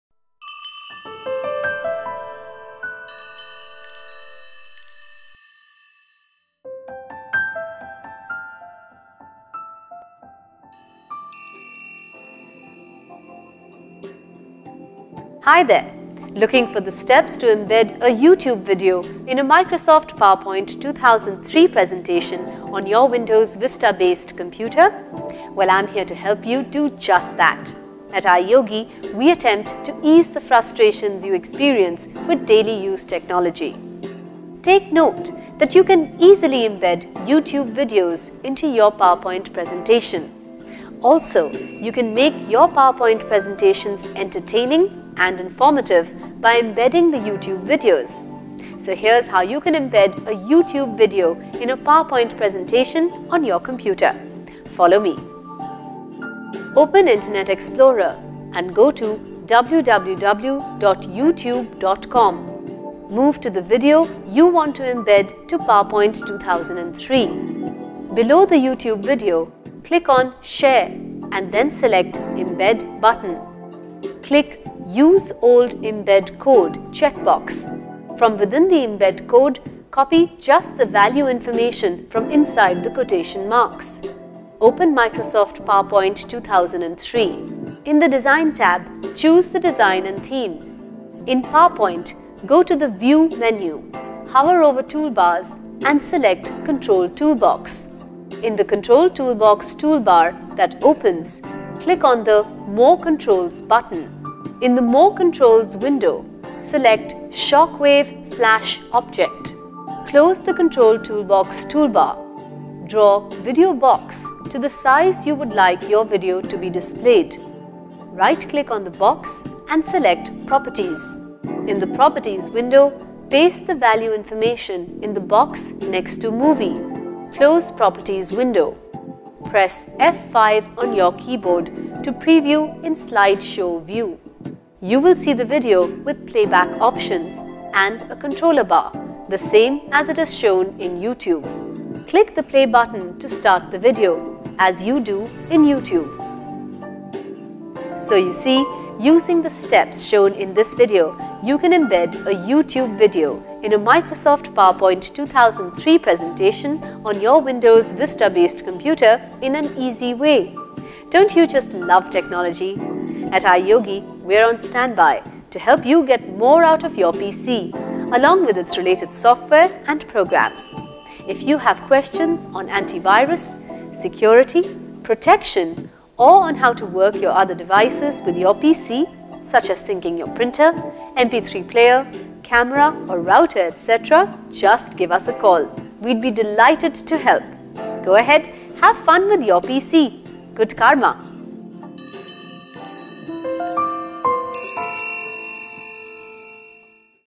PowerPoint 2003: How to embed a video presentation on Windows® Vista
If you are looking for the steps to embed a YouTube® video in Microsoft® PowerPoint 2003 presentation on a Windows® Vista-based PC. Take note that you can easily embed YouTube® videos into your PowerPoint presentation. Also, you can make your PowerPoint presentations entertaining and informative by embedding the YouTube® videos.